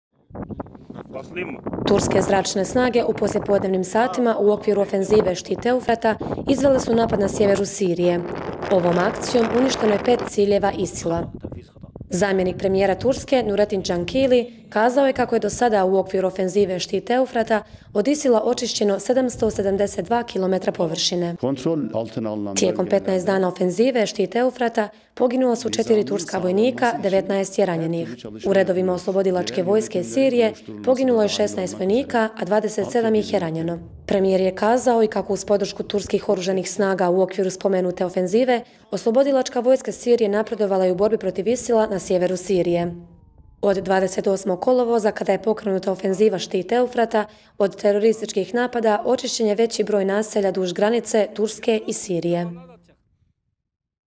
Turske zračne snage izvele napad na sjeveru Sirije
Turske zračne snage u poslijepodnevnim satima u okviru ofenzive „Štit Eufrata“ izvele su napad na sjeveru Sirije.